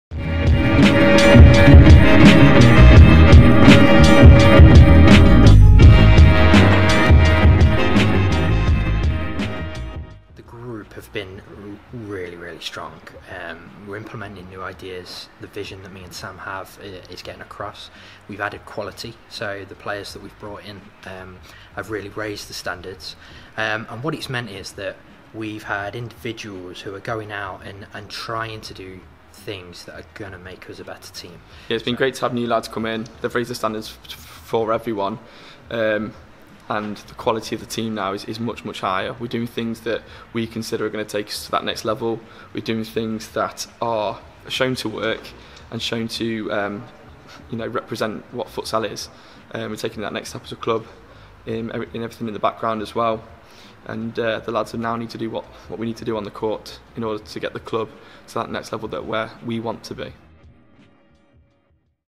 National Futsal Series | Derby Futsal Club speak about their new signings | 2019/20
Ahead of the inaugural National Futsal Series game in Durham Matt Hardy and Sam Huntingdon give their views on the new players in the squad.

derby_futsal